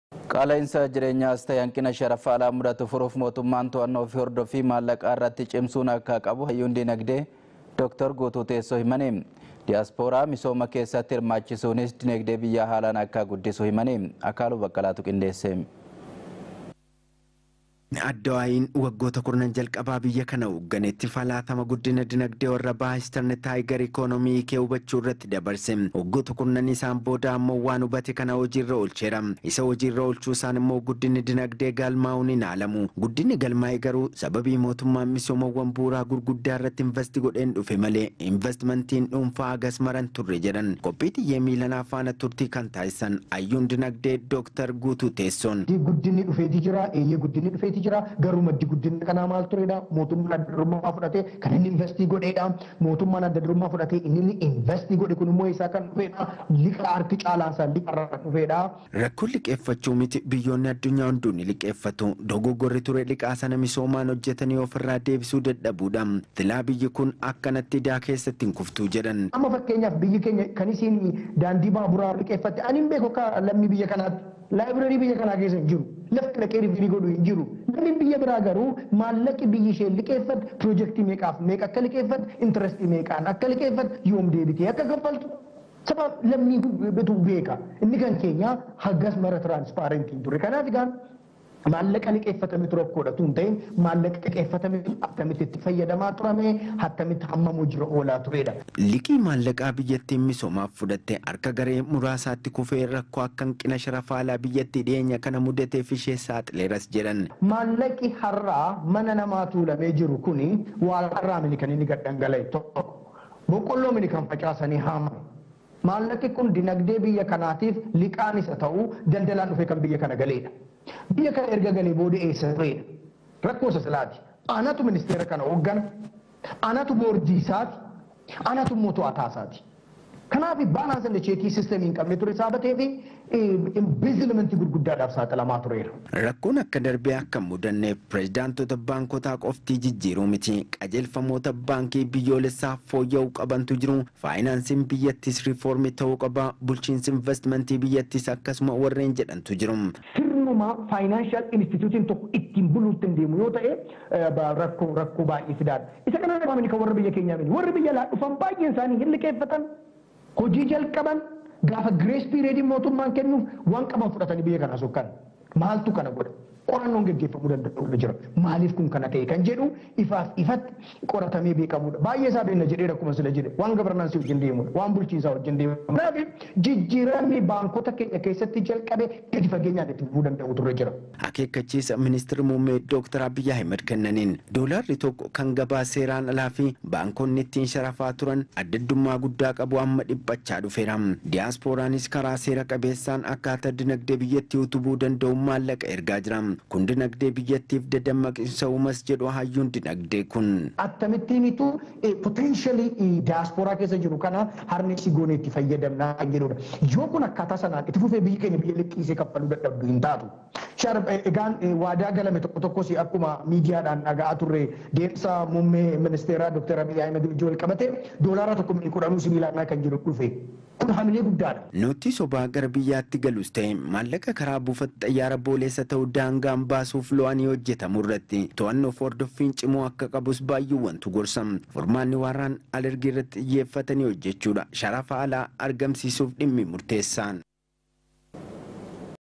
Qaala'insa Jireenyaa 24 11 2010
Hayyuu  Dinagdee
[OBN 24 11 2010] Qaala'insa jireenyaas ta’e hanqina sharafa alaa mudatu furuuf mootummaan to’annoo fi hordoffii maallaqarratti cimsuu akka qabu hayyuun dinagdee Dr.Guutuu Teessoo himan. Diyaaspooraa misooma keessatti hirmaachisuunis dinagdee biyyaa haalaan akka guddisus himan.